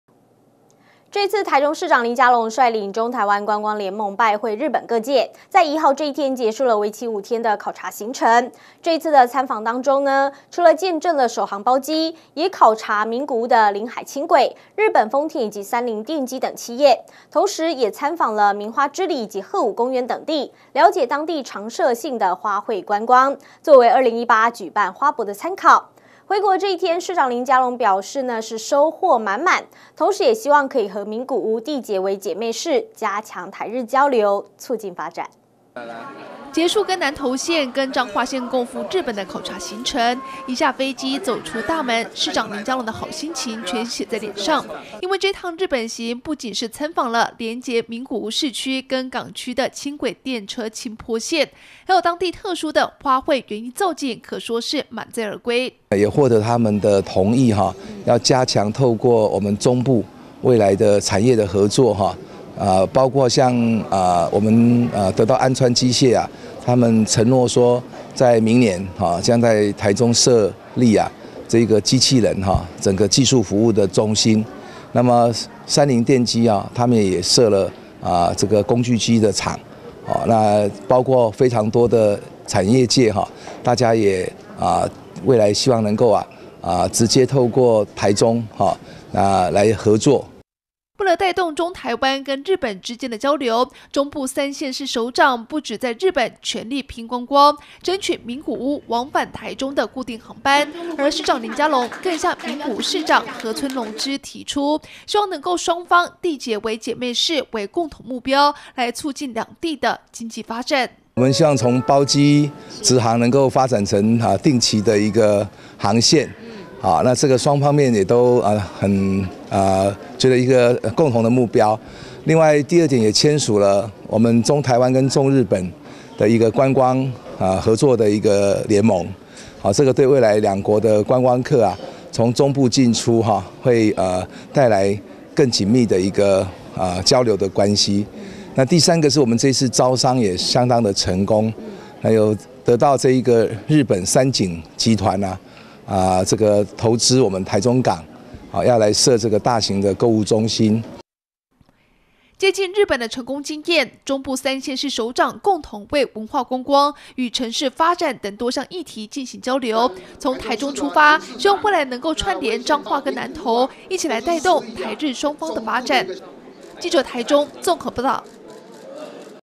大台中新聞 日本之行榮歸
這次台中市長林佳龍率領中台灣觀光聯盟拜會日本各界，在一號這天結束了為期五天的考察行程，在這次的參訪行程中，除了見証首航包機，也考察名古屋的臨海輕軌、日本豐田及三菱電機等企業，同時也參訪名花之里及鶴舞公園等地，了解當地常設性的花卉觀光，做為2018舉辦花博的參考，回國這天，市長林佳龍也覺得收獲滿滿，同時也表示希望能和名古屋締結為姐妹市，加強台日交流，促進發展。「以上新聞可從哈TV APP收視」